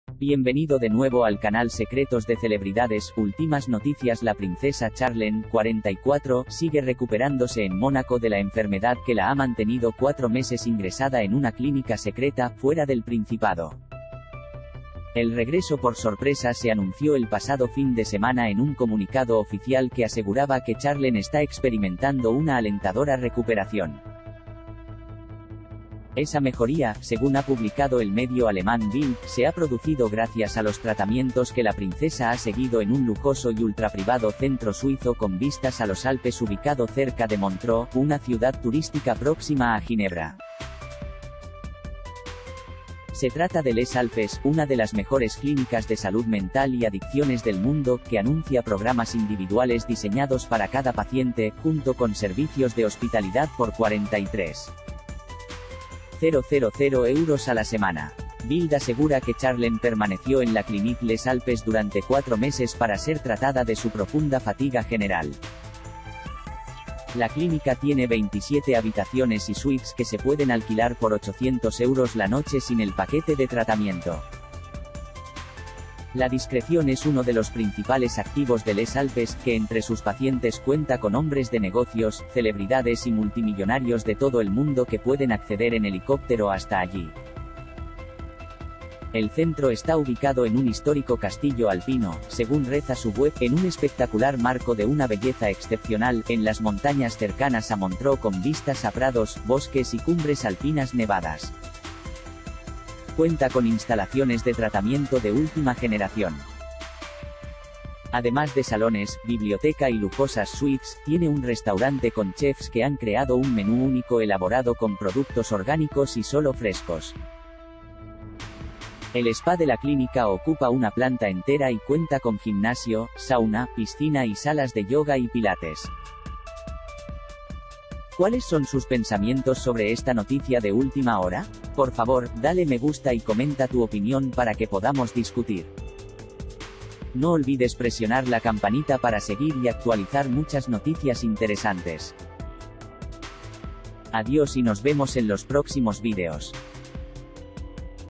43 000 euros a la semana y con vistas a los Alpes
43 000 euros a la semana y con vistas a los Alpes. The small four-star hotel, run by the Madreiter family, is located in the quiet center of Leogang, in the Salzburger Land. Under the motto LEBE FREI the guest can enjoy his vacation in the hotel free of the constraints of the everyday life. With the LEONARIUM, the host family Madreiter makes a clear statement for more enjoyment of life. The modern, natural rooms have wooden floors and a restful sleep system.